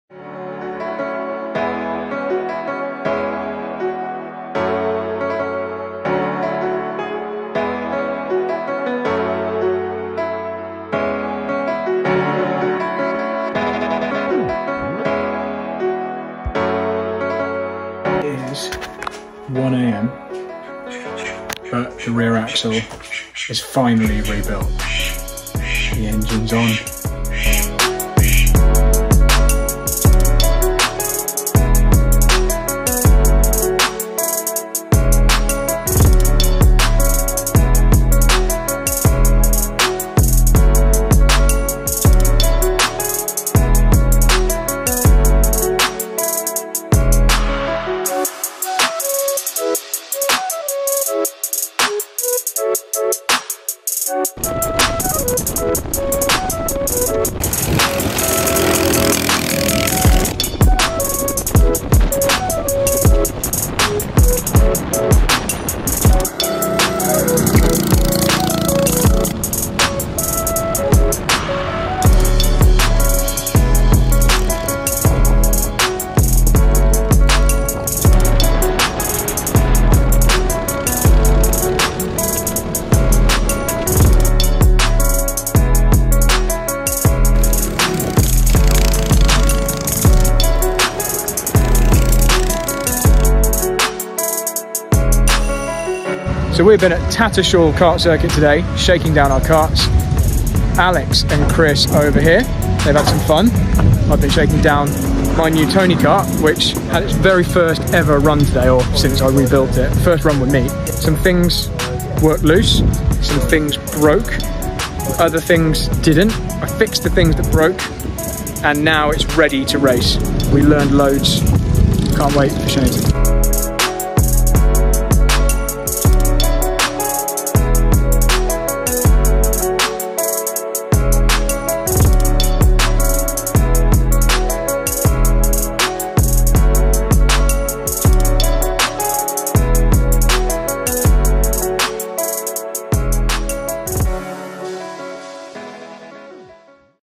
Will It Break? | First Laps In My New GX-UK Kart!
Over the last three weeks, I’ve been building my 2018
Tony Kart ready for the second GX-UK race of the year.

In this video, I take the kart for its first shakedown at Tattershall - a circuit I’ve never been to before, but turned out to be the perfect test track, with a great combination of corners.